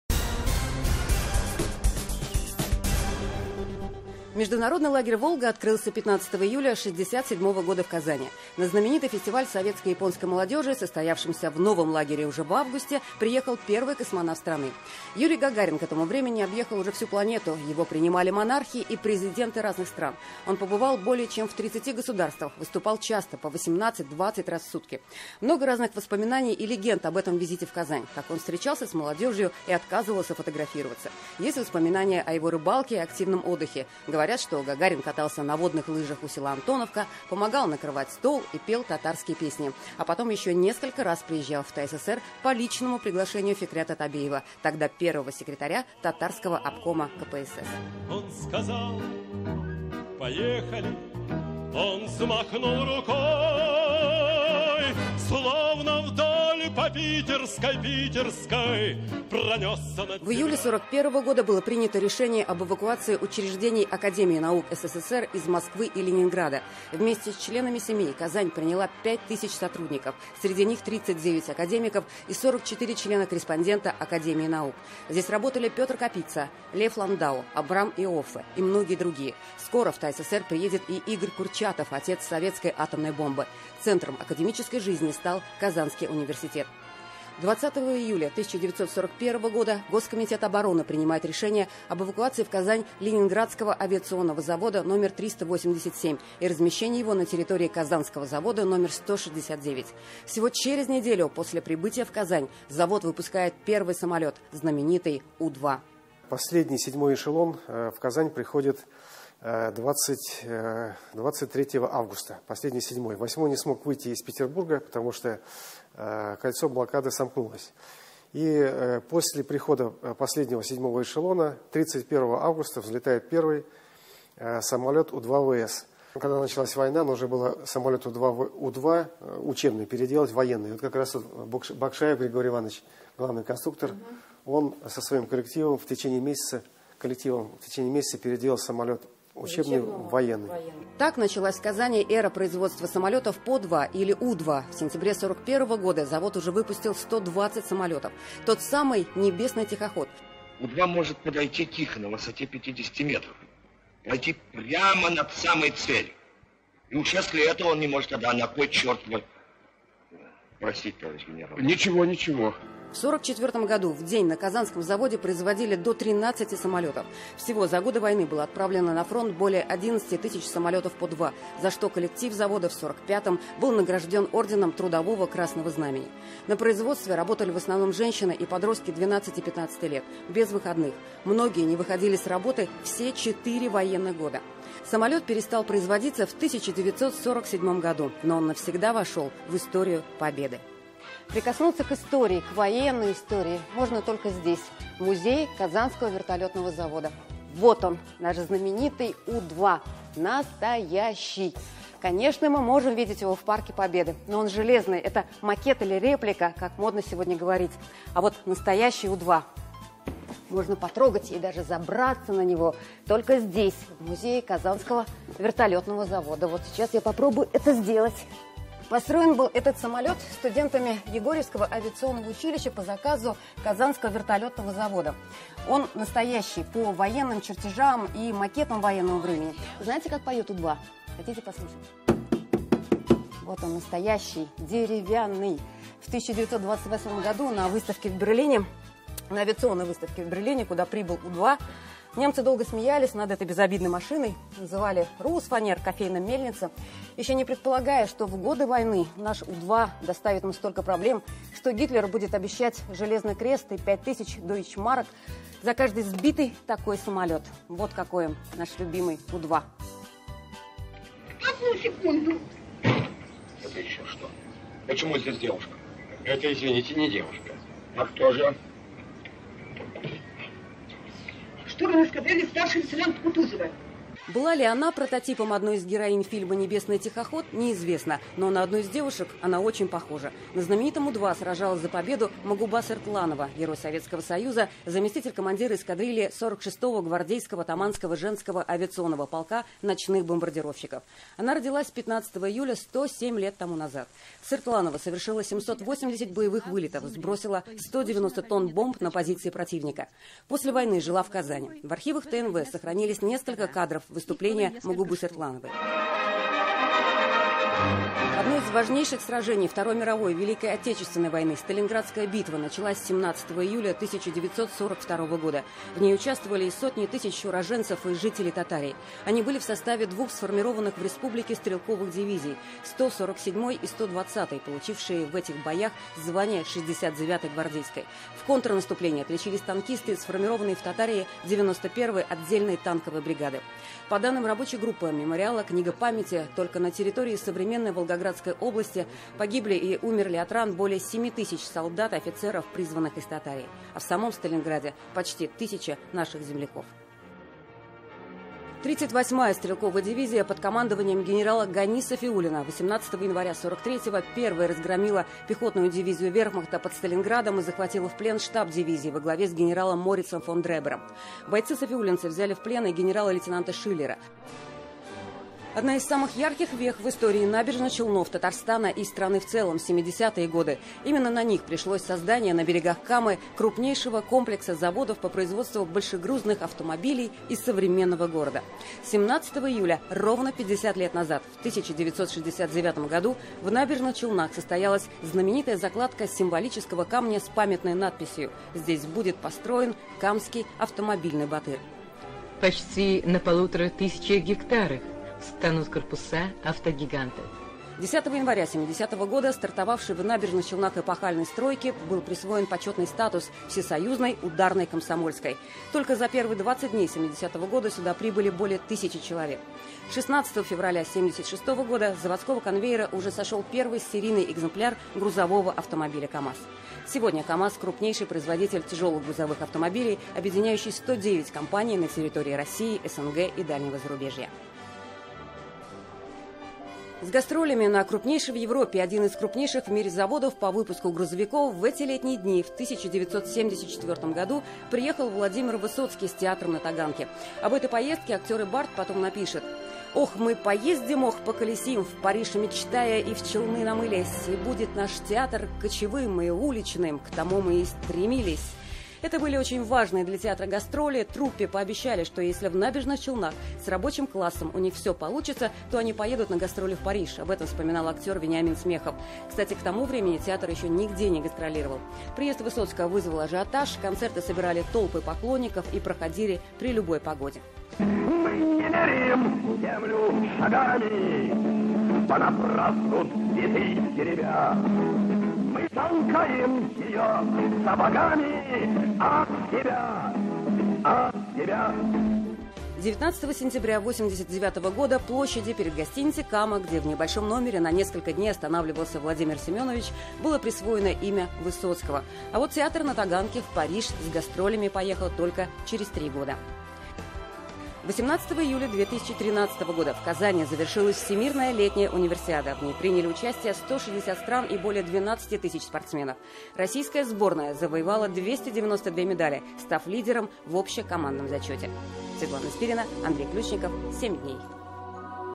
100летТАССР Неделя в столетии. 7 дней | ТНВ
Наша традиционная рубрика «Неделя в столетии», посвященная 100-летию ТАССР. Люди, события, факты – определившие развитие Татарстана – в репортаже Светланы Спириной.

Координаты:
☎ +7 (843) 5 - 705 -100